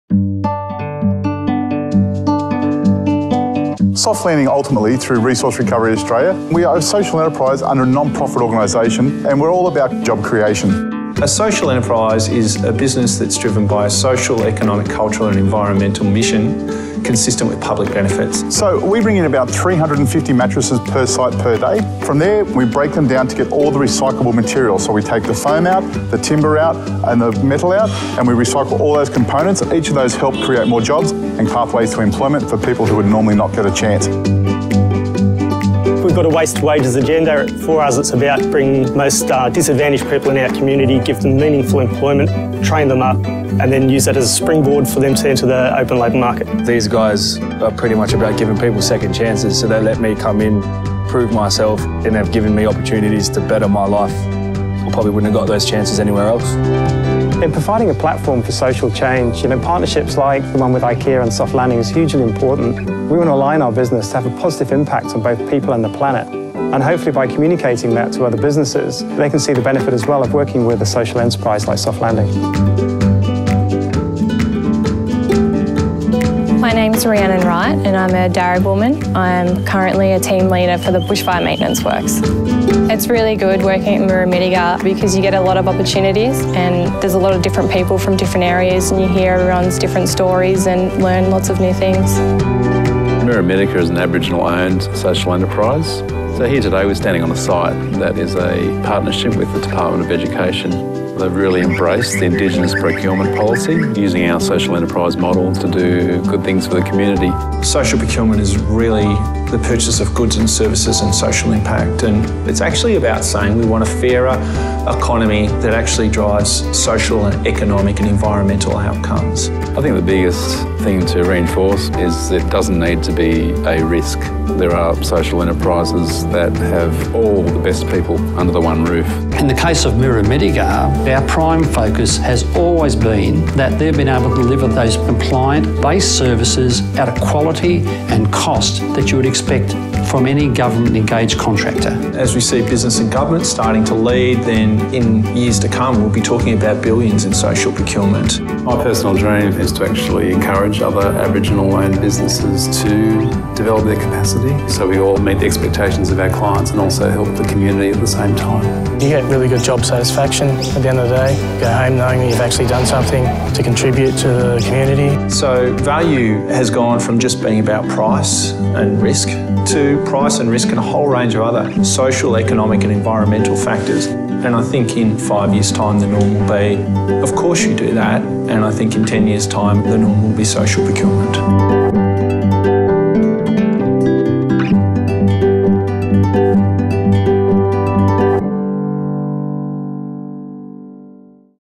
Social Enterprise: Good for business, good for community
Social enterprises offer professional goods and services with the added value of social and environmental benefits –  learn about buying better.

environment.nsw.gov.au/communities